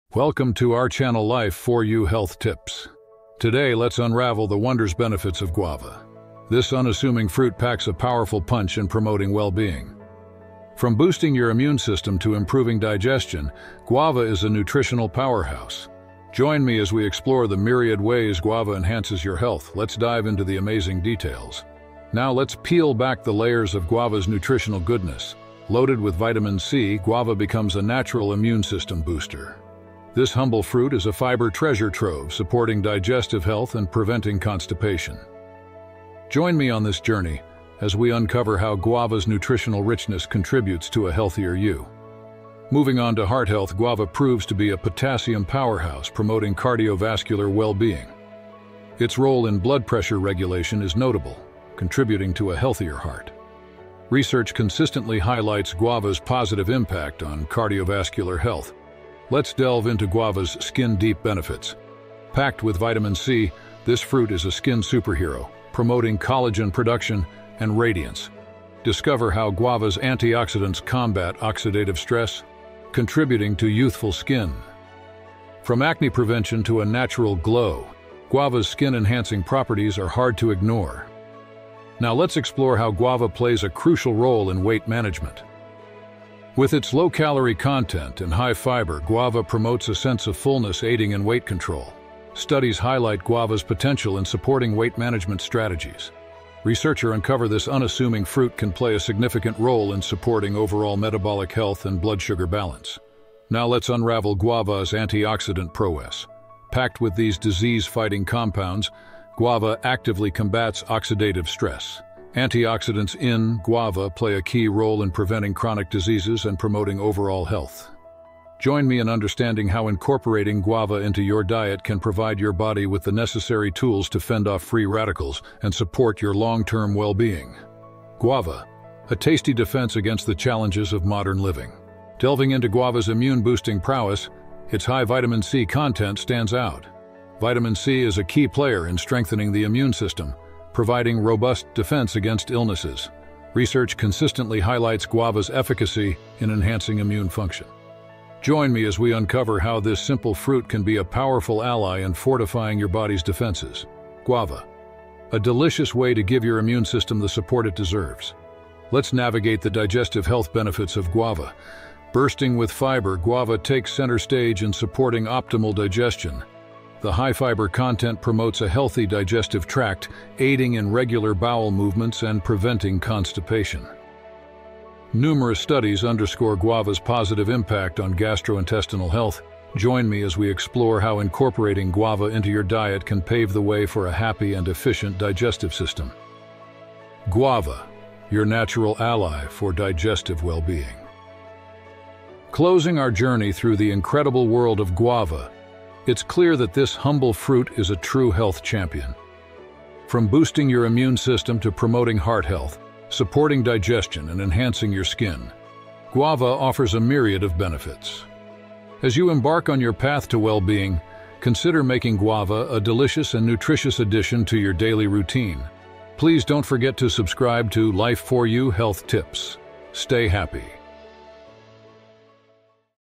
Nourishing Your Well-being: The Guava Revolution | Life4U-Health Tips
Welcome to Life4U-Health Tips! Join us on an enlightening journey through the incredible world of guava, the unassuming fruit with a powerhouse of health benefits. Discover the wonders of guava, from boosting your immune system to promoting heart health, supporting digestion, and enhancing your skin. Unravel the nutritional goodness and learn how guava can be a delicious and nutritious addition to your daily routine.